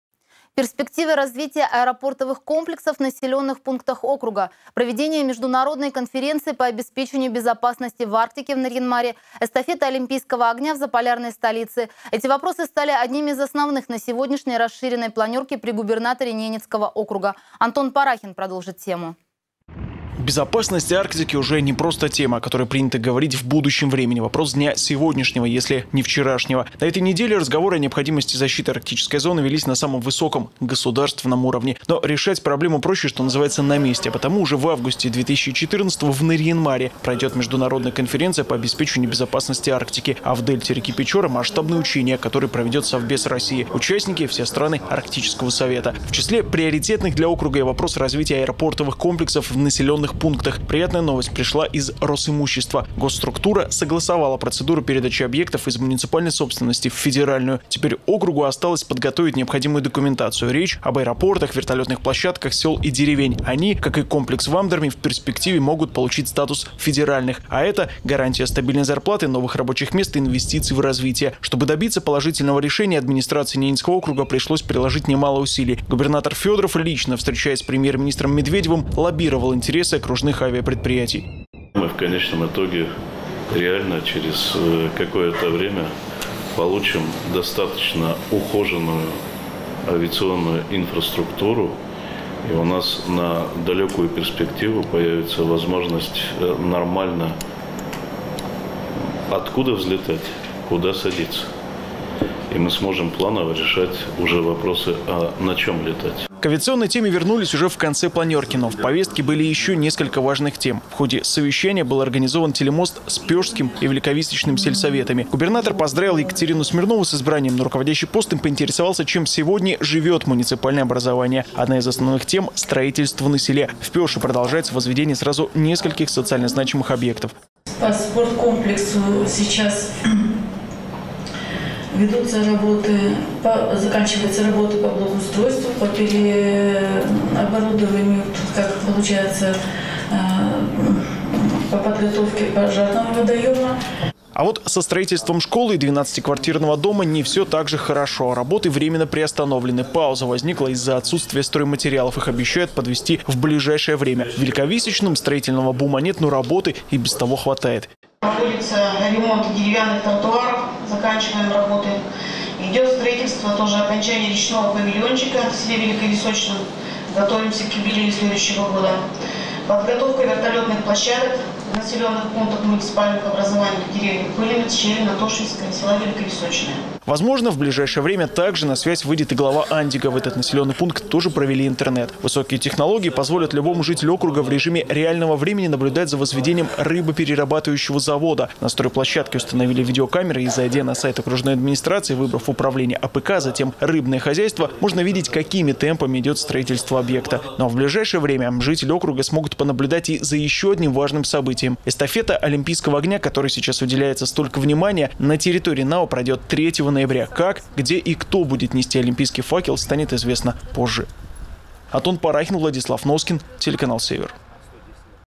Безопасность, развитие, престиж. Актуальные вопросы обсудили на планерке при Губернаторе
Перспективы развития аэропортовых комплексов в населенных пунктах округа, проведение международной конференции по обеспечению безопасности в Арктике в Нарьян-Маре, эстафета олимпийского огня в Заполярной столице - эти вопросы стали одними из основных на сегодняшней расширенной планерке при Губернаторе Ненецкого округа.